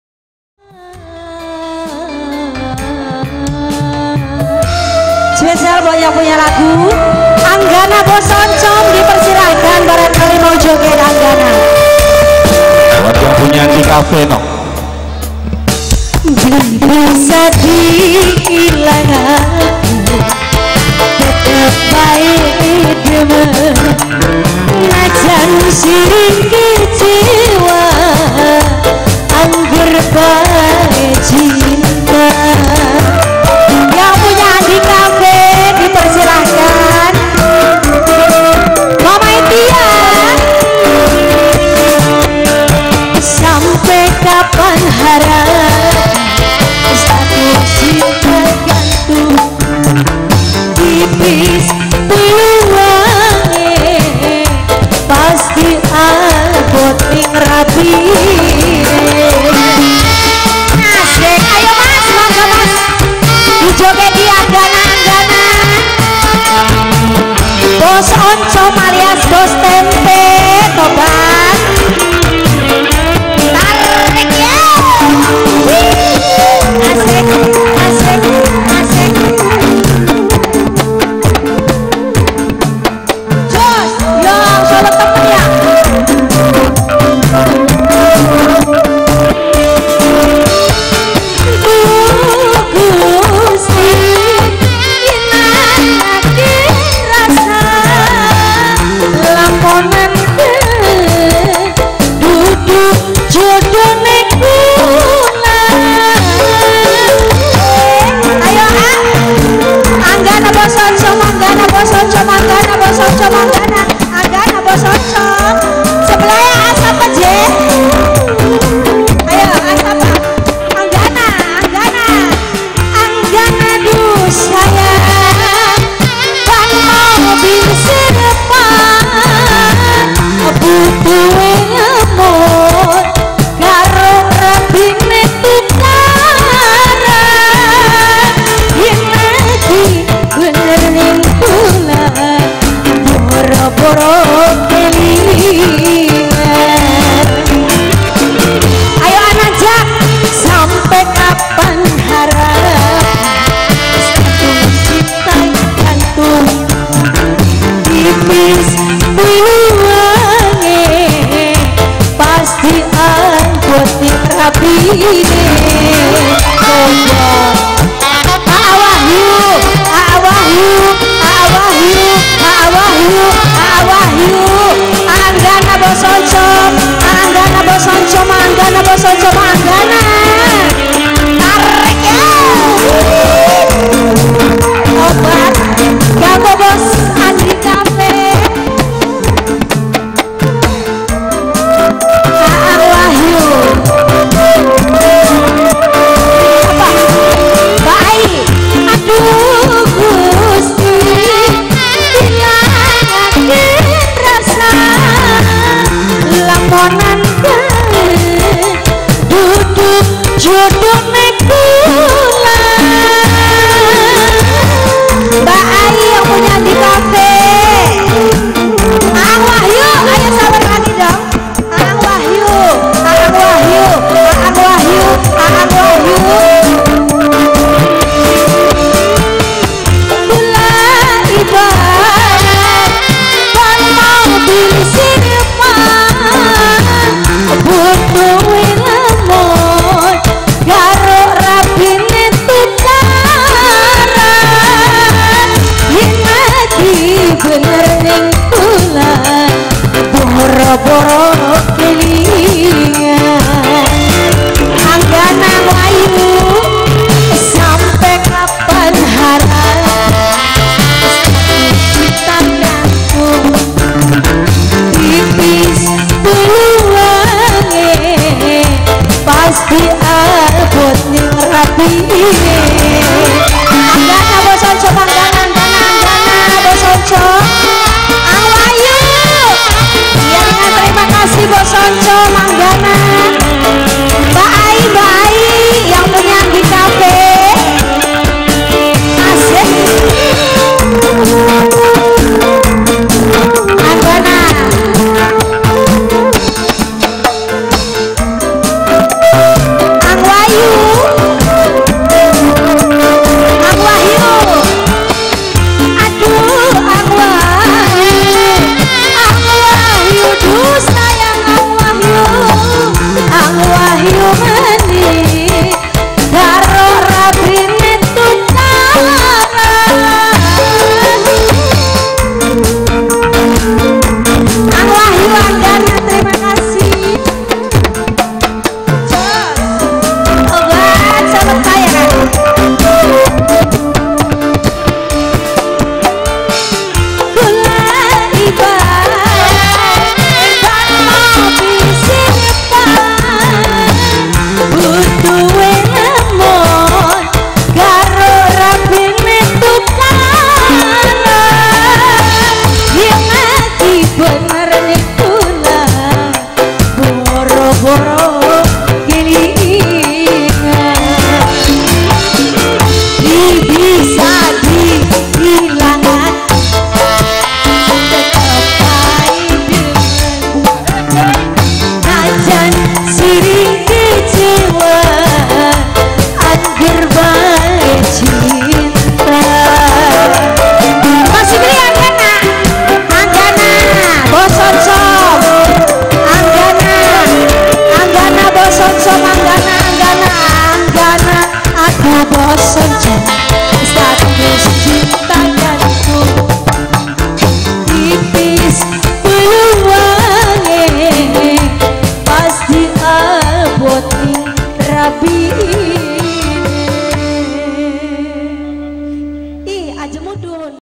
BAN SEREP / NADA ZALFANIA/ TERUSAN /TANGGAL 17 FEBRUARI 2022/
Hajatnya kel Bpk Junedi / Ibu Umamah , Desa Terusan , Khitanan Muhammad Abbie Prayoga
** DEA PRODUCTIONS** Photography & Video Shoting , Live Streaming  Alamat Desa  Kongsijaya Gang Purbaya , Kec .